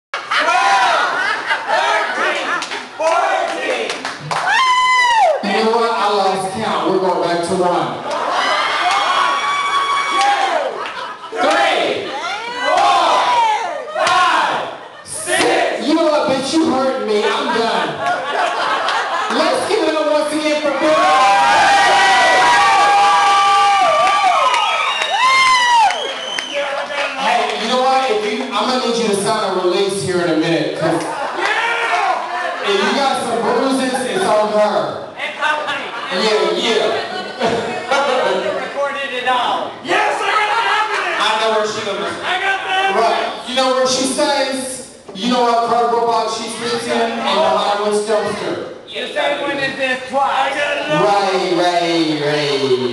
China Blue Spanking bday boy
China Blue spanking a birthday boy at The 220.